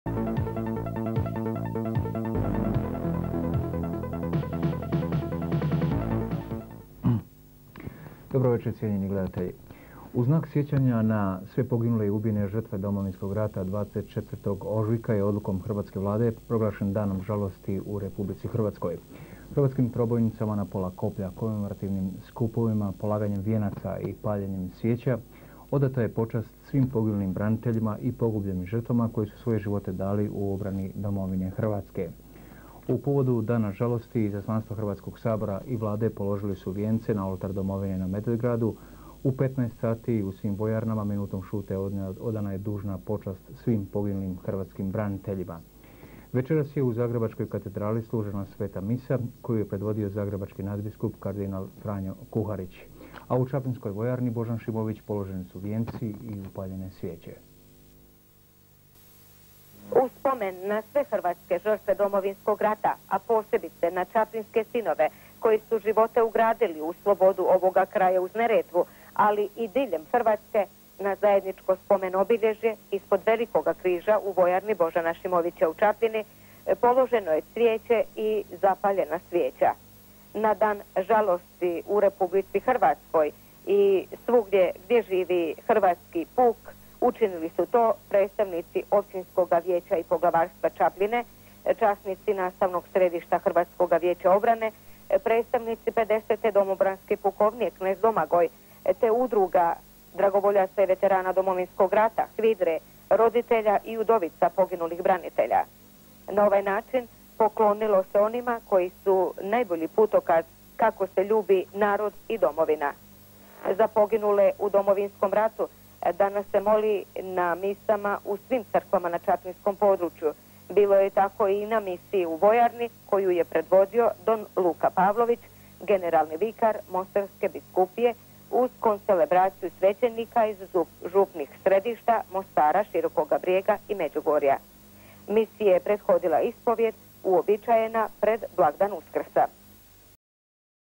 HTV MOSTAR - IZVJESTAJ IVANKE DZAJIC IZ CAPLJINE 1997.
Volio bih da pažljivo pogledate i poslušate ovaj izvještaj iz Čapljine. Riječ je o javljanju čapljinske novinarke Ivanke Džajić za HTV Mostar 1997. godine.